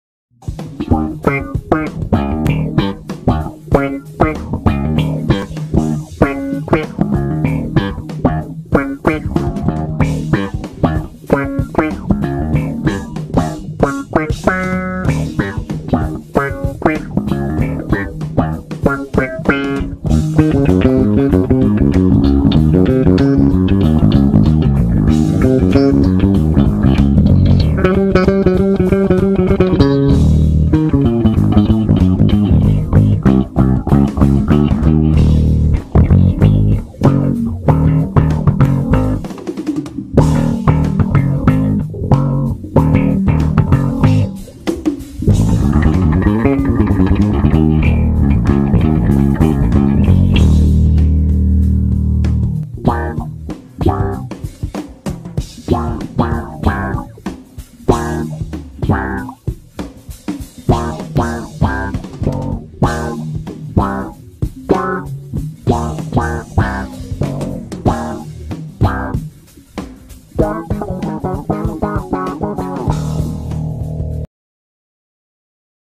Flea Master Session - Bass Solo with Envelope Filter
Gear used:
E.B MusicMan Stingray

MXR Envelope Filter
EBS Multicomp

Harbinger 502 mixer